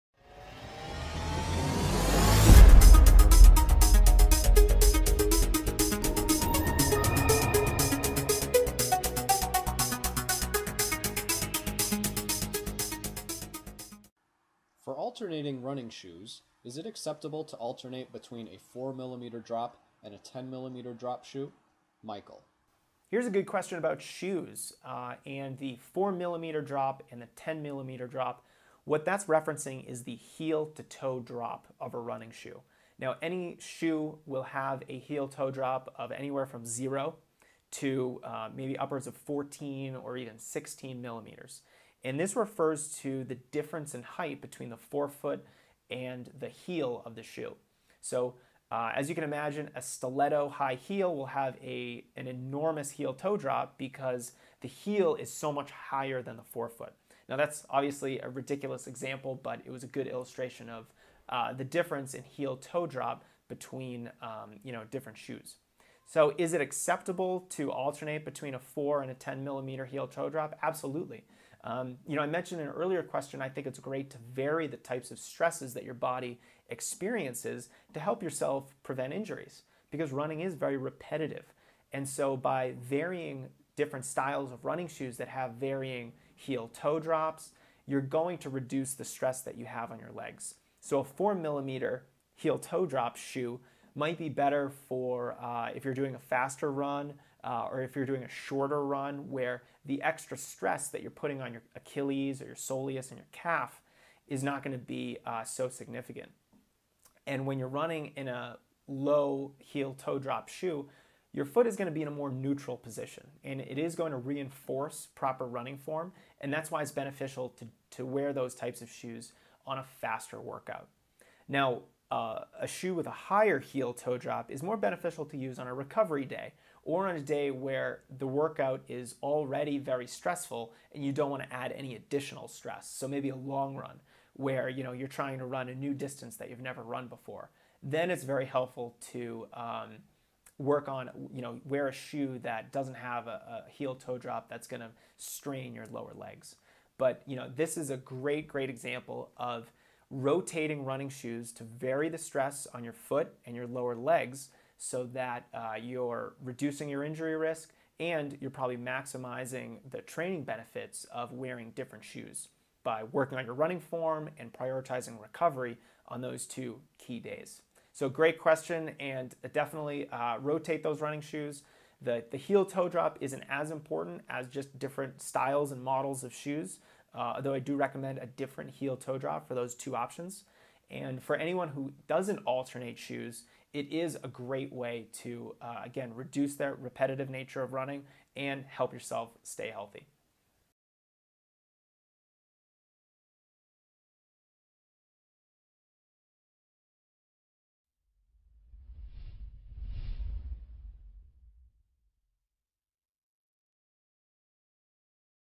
Should I rotate my running shoes?
Rotating your running shoes is a great way to vary the stress experienced by your feet and lower legs. Watch this video to learn about heel-toe drop, running shoes, and more.